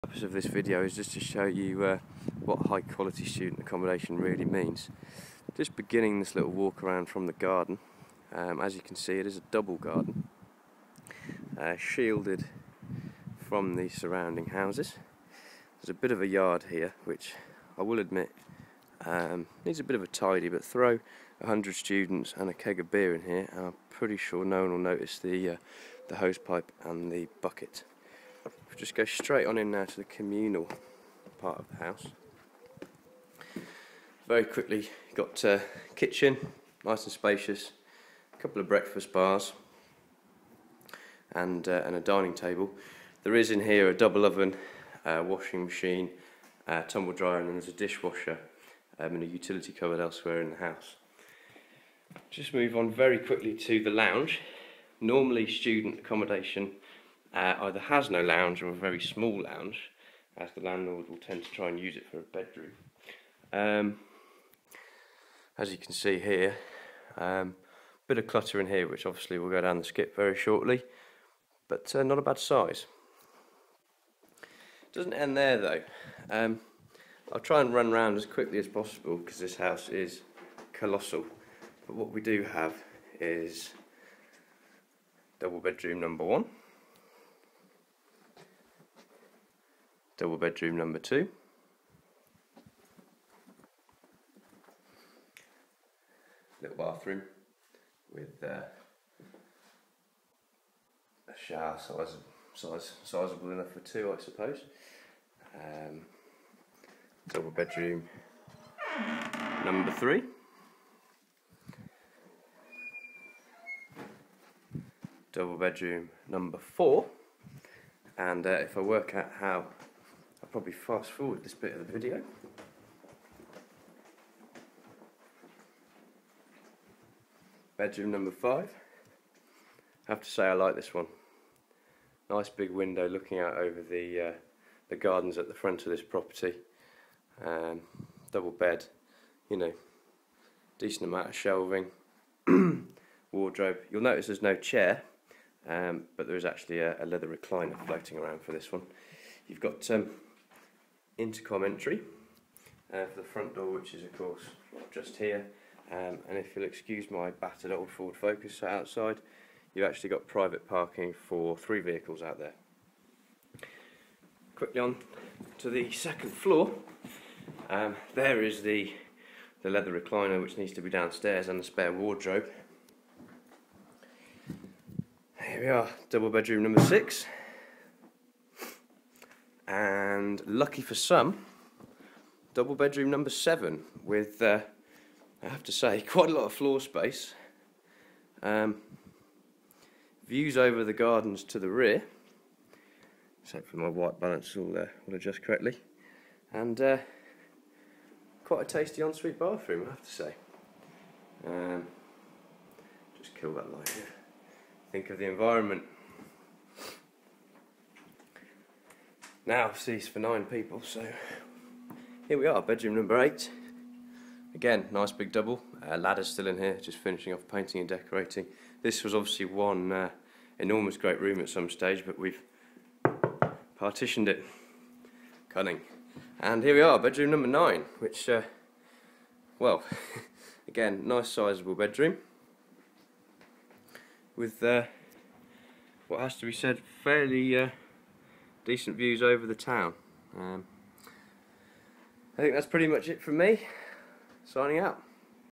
Smarter Housing | Student Accommodation | Leamington Spa (9 bedrooms)
Smarter Housing - 'raising the standard' of student accommodation, residential letting and property sales.

This video tour shows a luxurious 9 bedroom apartment on Clarendon Square in Leaminton Spa, Available as a student accommodation, the property features 9 double bedrooms (one with en-suite bathroom), spacious kitchen and diner, separate lounge and utility room.

There is private parking for two vehicles to the front and a spacious double garden (and yard!) to the rear.

Rent includes all bills [fair usage policy applies] and broadband internet.